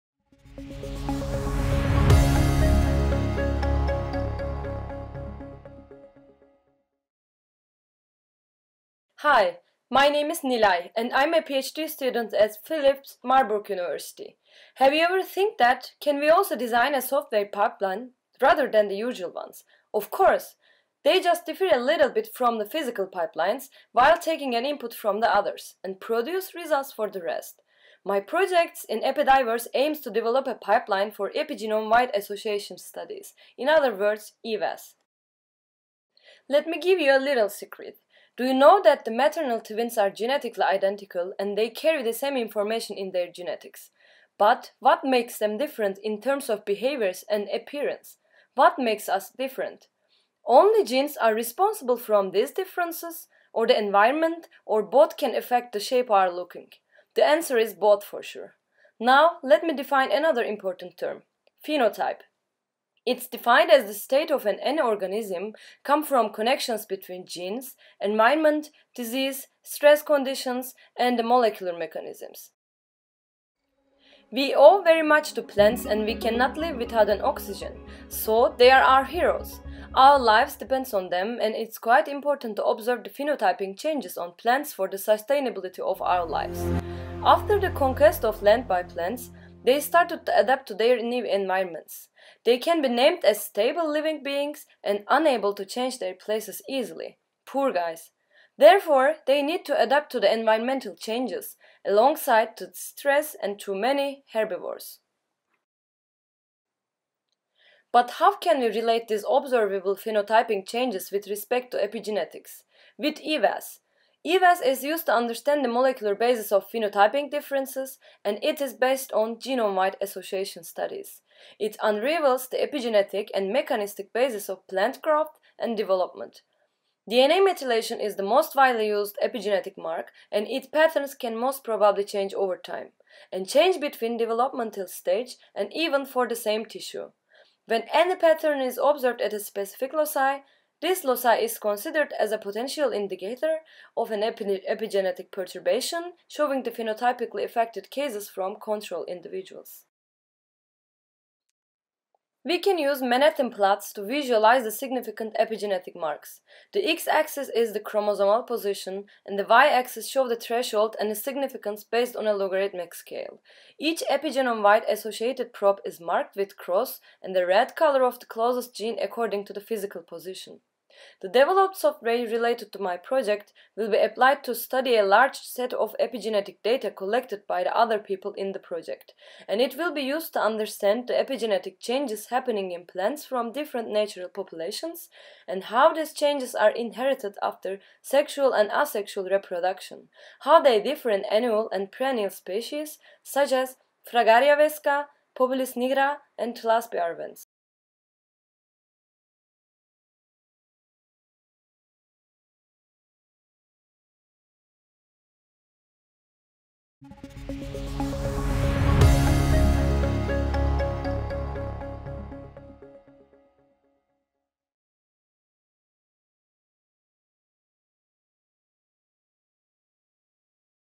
Epigenome-wide association studies
Nilay is developing a pipeline for Epigenome-wide association studies (EWAS) at the Philipps-University in Marburg, Germany. Her software will be applied to study a large set of epigenetic data produced to understand the epigenetic changes happening in plants from different natural populations, how these changes are inherited after sexual and asexual reproduction and how they differ in annual and perennial species such as Fragaria vesca (strawberry) and Populus nigra (black poplar).